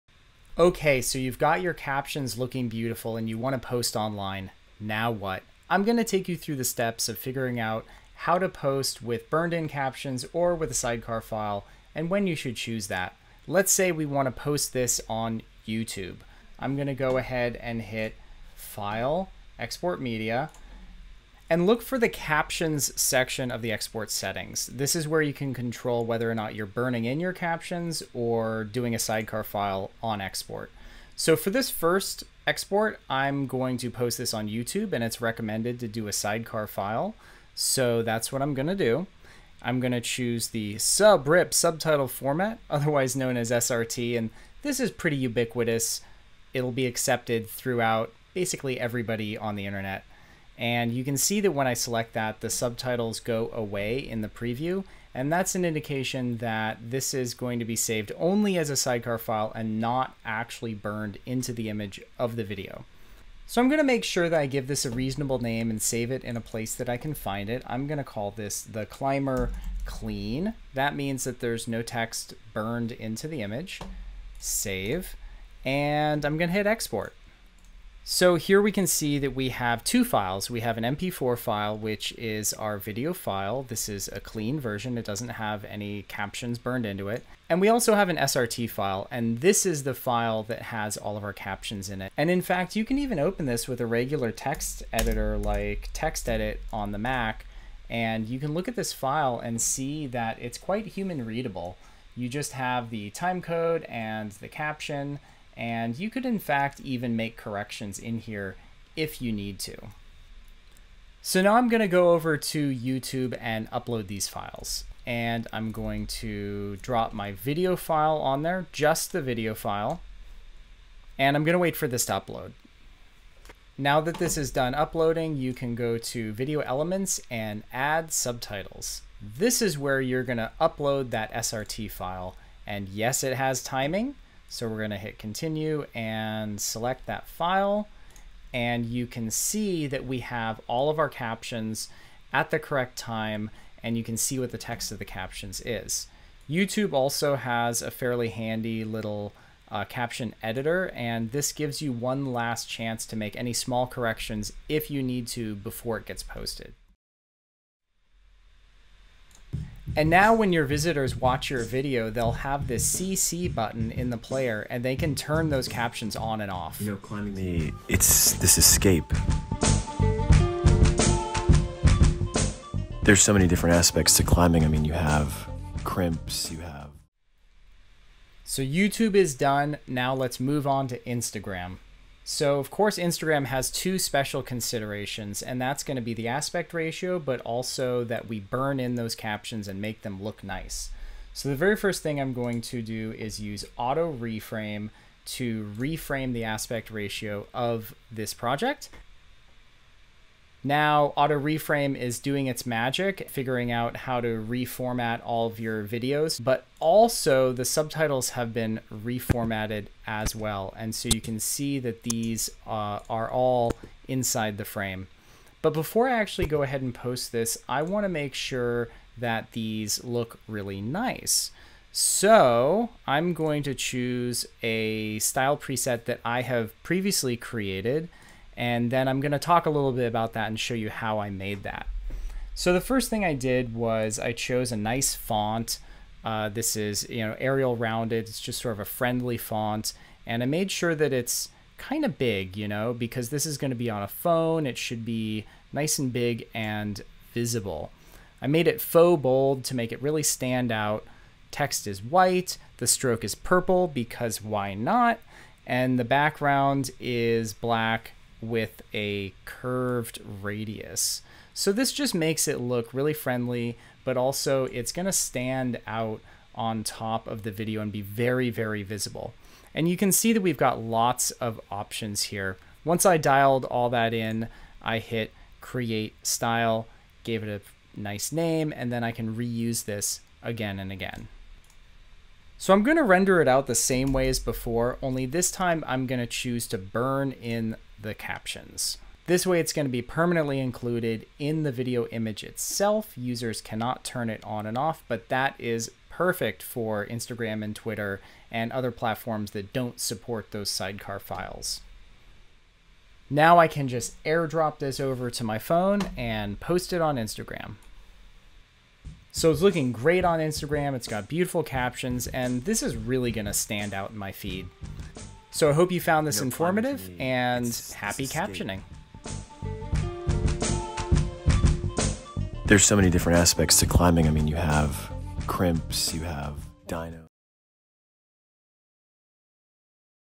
How to export captions and .SRT files with Premiere Pro Product Manager Francis Crossman
Premiere Pro Product Manager Francis Crossman is here to give you a walkthrough of how to export captions, subtitles, and sidecar files. With Premiere Pro's new caption workflow, you can make your videos more accessible and easier to find across Youtube and all of social media. It's easier than it sounds, promise! 😁

0:00-1:52 Exporting. SRT files
1:52-2:57 Uploading .SRT files to Youtube
2:57-3:34 Auto-reframe captions for social media
3:34-4:40 Create style presets for captions
4:40-5:27 Export settings for social media